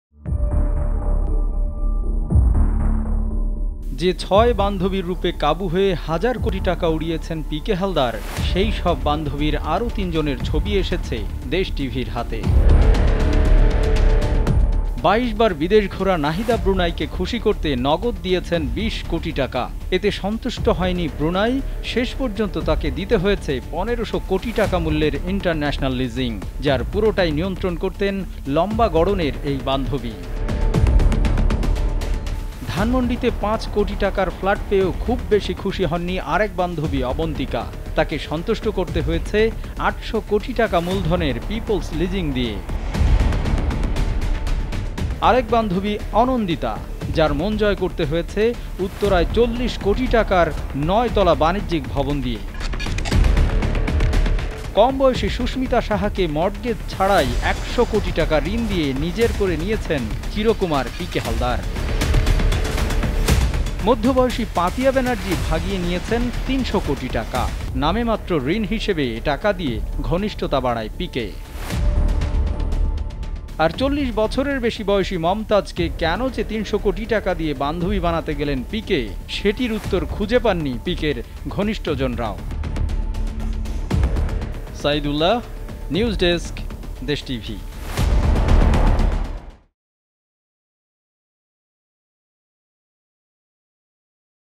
বেরিয়ে আসছে থলের বিড়াল, পিকে’র আরো নতুন বান্ধবীর সন্ধান! | PK_Halder | PK's_Girlfriend |6_Girlfriend
বেরিয়ে আসছে থলের বিড়াল, পিকে’র আরো নতুন বান্ধবীর সন্ধান! |

»» Office Address:
42, Shaheed Sangbadik Selina Parveen Sarak, Malibag 1217 Dhaka, Dhaka Division, Bangladesh